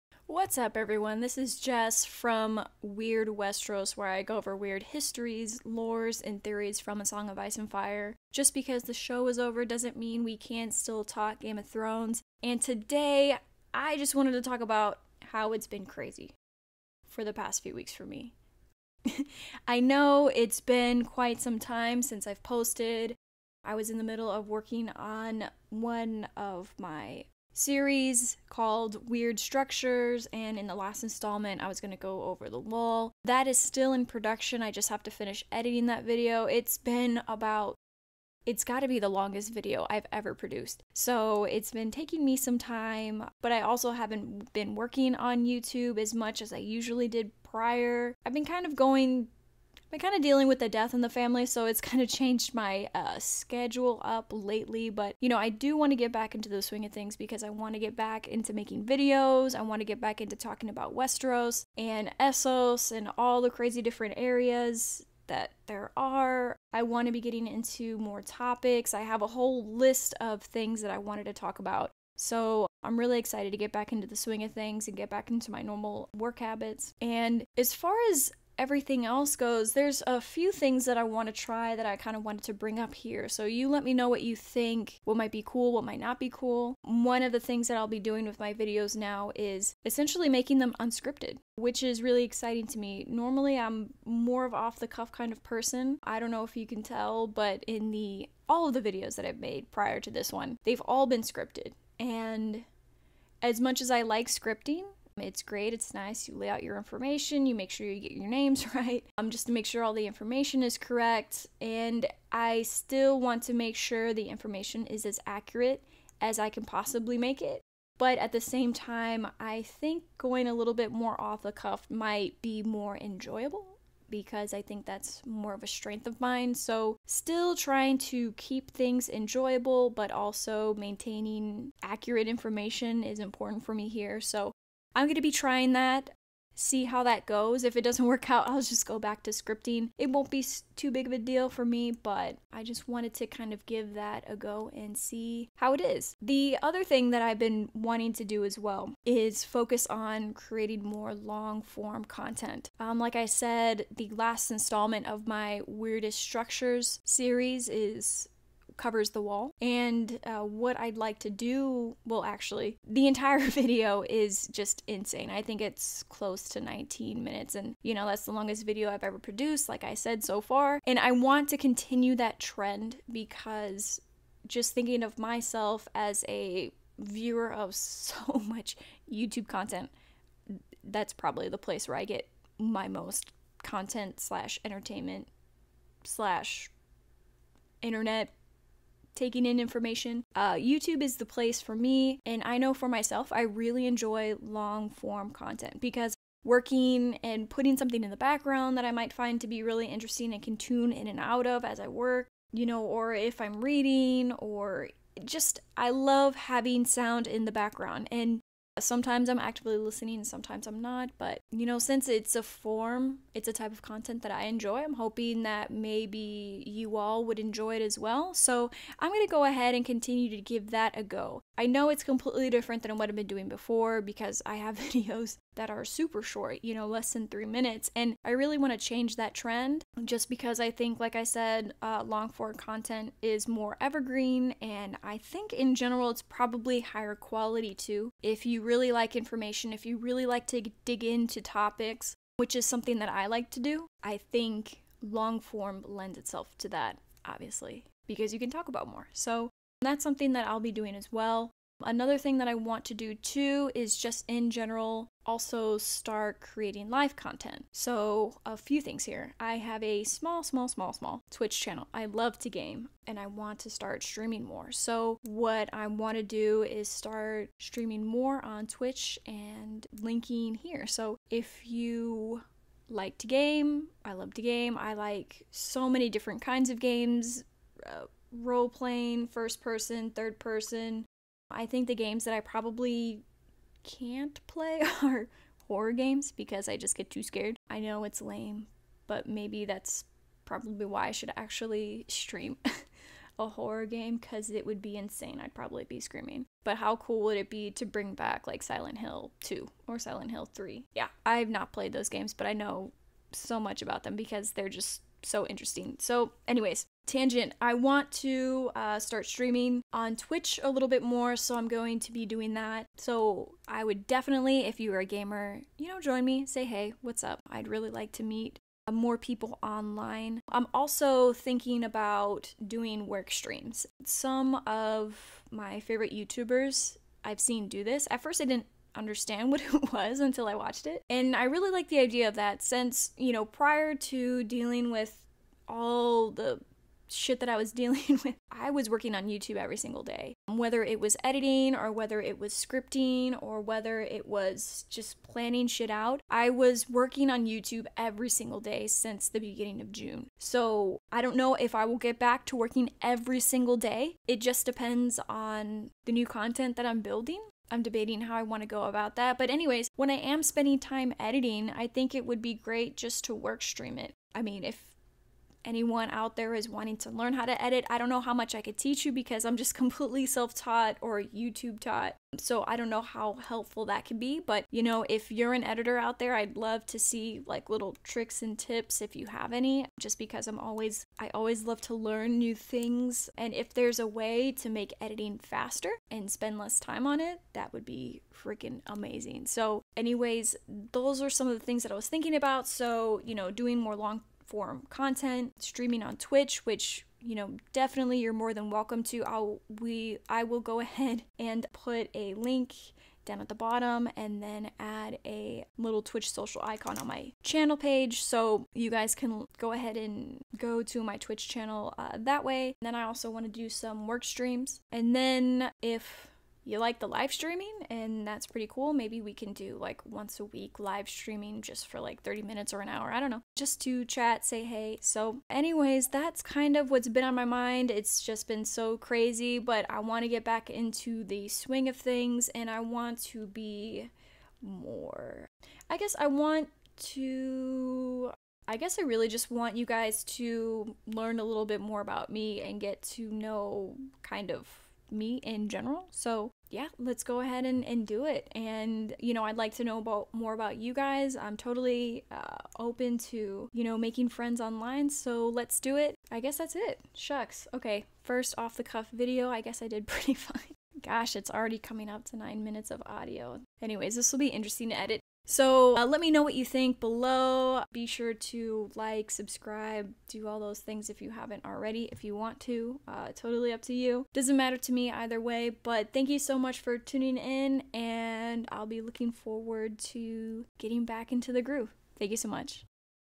I'm ashamed to say it, but things have been a bit crazy for me
Hello everyone! This is Jess from Weird Westeros and welcome! Here, I go over weird histories, lores and theories from a Song of Ice and Fire and today I wanted to break up my hiatus.

Yes I've been gone for a few weeks due to a death in the family, but now I'd like to get back into the swing of things.

The plan is to create more new content and finish my Weirdest Structures Series. Here are some things to expect moving forward:
- Work streams when I'm editing
- Twitch streaming since I love  playing video games
- Long form, unscripted content

Just some things I want to try out :)

Let me know if you have any suggestions or requests below. I'm sorry for the hiatus, but I'll be back to our normal scheduled programming. Thanks everyone!